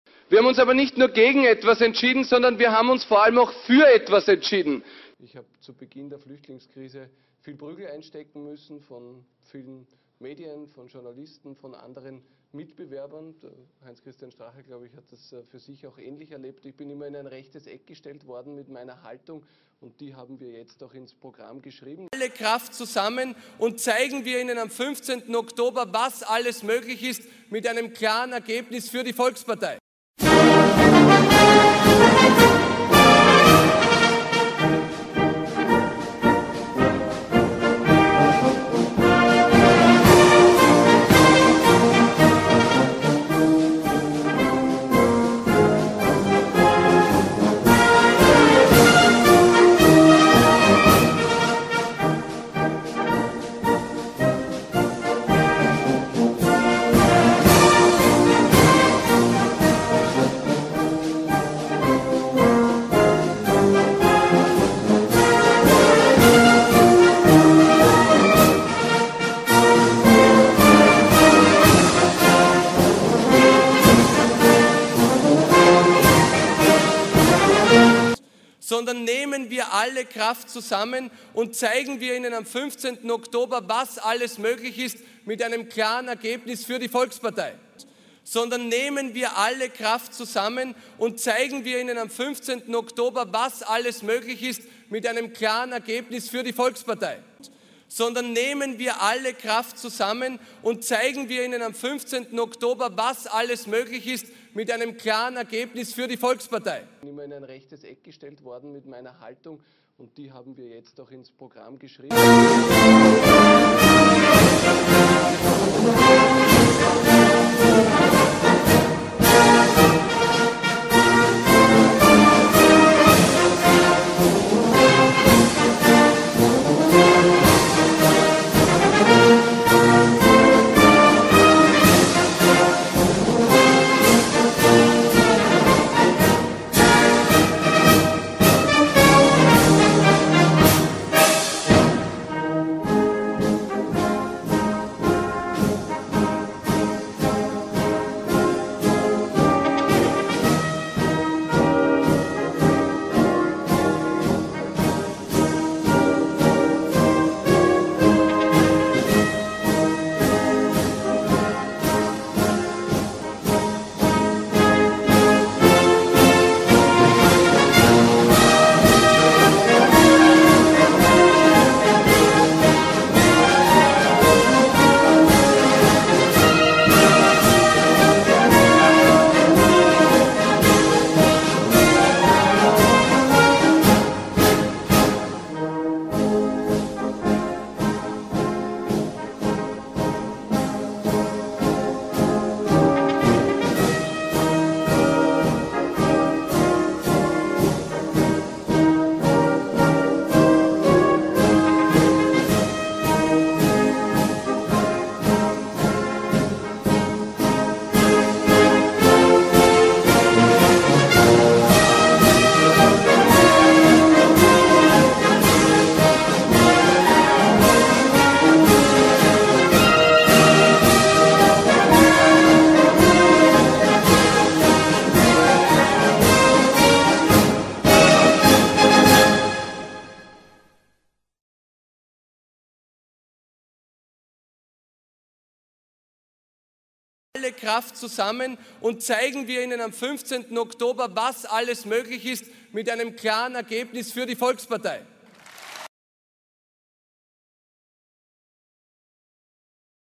Sebastian Kurz - Rechte Gesinnung ins Parteiprogramm geschrieben - Achtung 6"-20" sehr leise
Ehrliche Momente von Sebastian Kurz  -  man muss nur genau hinhören, oder auch sehen was seine Kollegen und Förderer tun.

LH Platter weigert sich auf der Homepage der ÖVP den Eintrag zu Kurt Gattinger anzupassen und mit einem Hinweis zu versehen.

Khol startet einen Angriff auf die Pressefreiheit um den Groerskandal zu vertuschen.

Schönborns Haltung zu Groer ist bekannt - es gibt genügend dazu hier auf Youtube.

Dr. Johann Kurz Großonkel von Sebastian Kurz
Tiroler Blasmusik mit Nazimarsch in München